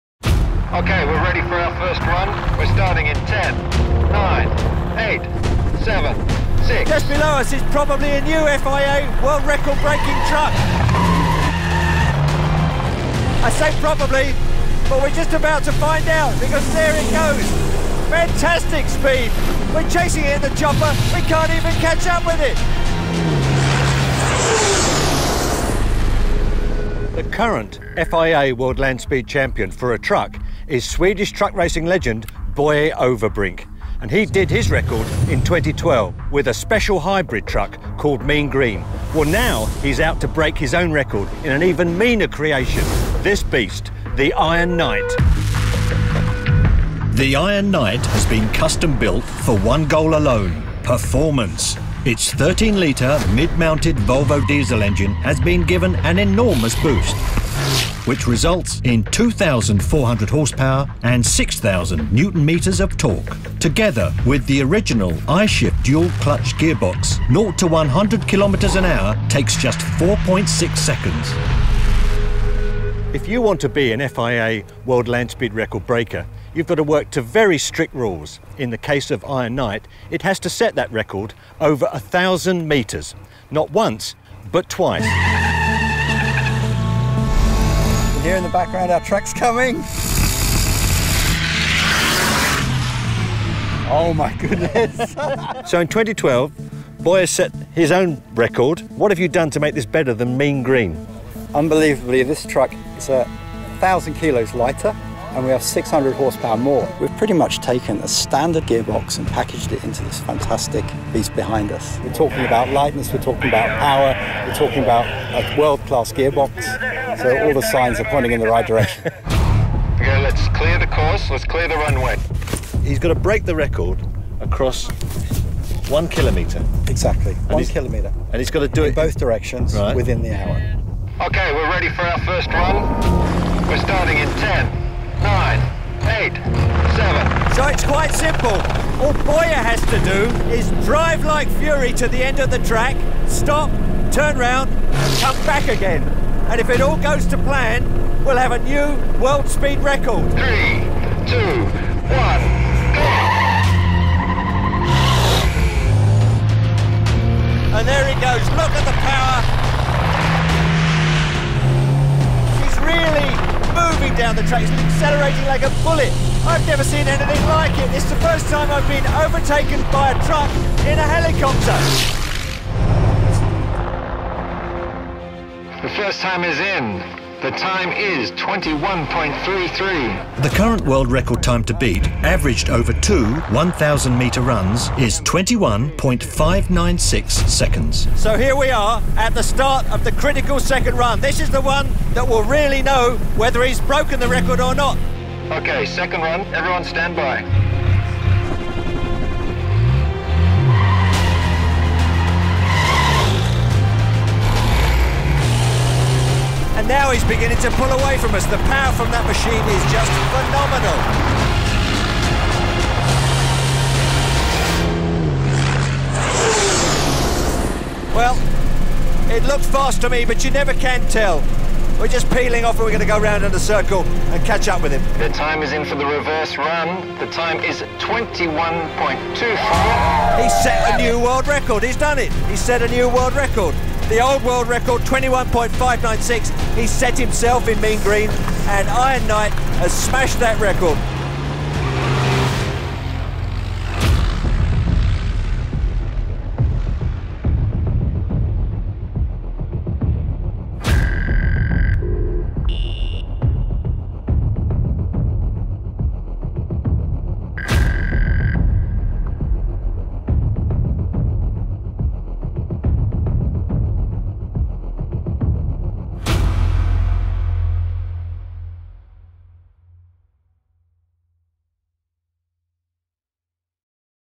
Volvo Trucks The Iron Knight takes down two world speed records
Volvo Trucks’ The Iron Knight is the world’s fastest truck – twice over. It has beaten the speed record in the standing-start 500- and 1000-metre categories. The record-breaker is custom-built in every respect apart from the engine and the series-manufactured I-Shift Dual Clutch transmission.

With an average speed of 169 km/h and a time of 21.29 seconds, The Iron Knight beat the international speed record for 1000 metres from a standing start. It also beat the corresponding record for the 500-metre distance, at 131.29 km/h and 13.71 seconds.